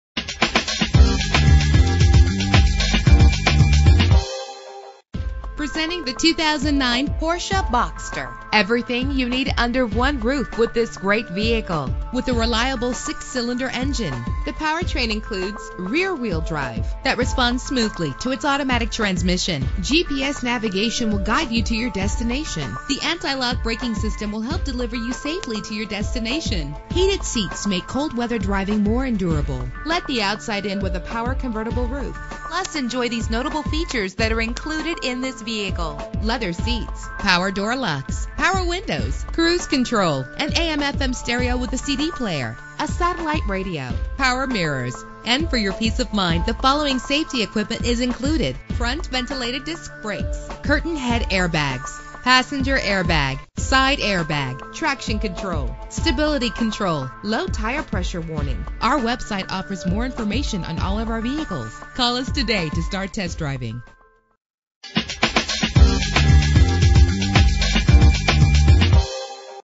2009 Porsche Boxster - Stafford TX
Year: 2009
Make: Porsche
Model: Boxster
Trim: S
Engine: 3.4 liter H6 24 valve
Transmission: Automatic
Color: Arctic Silver Metallic
Mileage: 33096
Address:
12053 Southwest Freeway